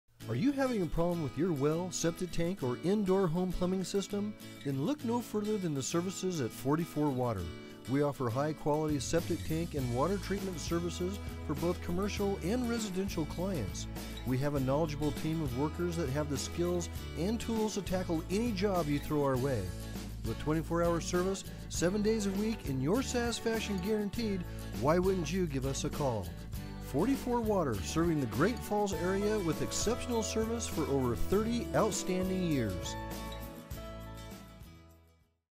Best Water Treatment Services Great Falls, MT
Best Water Treatment Services Great Falls, MT Our water services include Water Well Drilling, Pump Service, Filters, Water Cisterns, Septic Tanks, and more.

Proudly serving the Great Falls and Helena area for over 30 years.